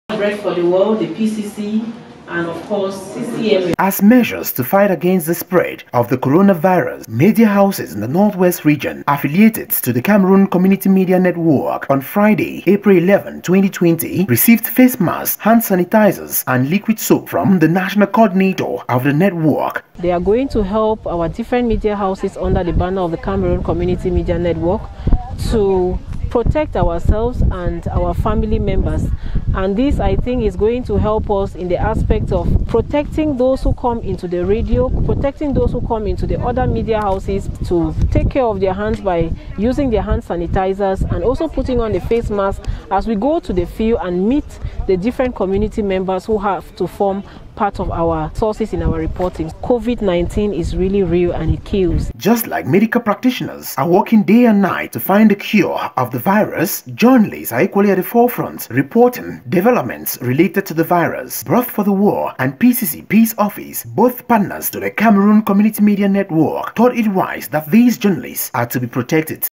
MEDIA HOUSES IN THE NORTH WEST RECEIVE FACE MASKS AND HAND SANITIZERS
Media houses under the banner of the Cameroon Community Media Network has received face masks, hand sanitizers and liquid soap this to help protect them against the Corona virus.